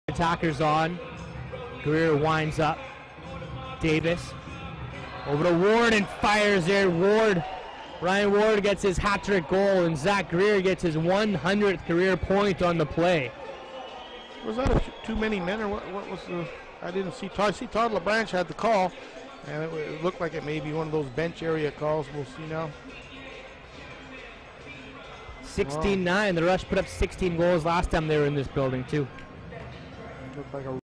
Ward gets hat trick by scoring on delayed penalty. 16-9 Rush.
Edmonton Rush Ward gets hat trick by scoring on delayed penalty. Increasing the lead to 16-9 Rush.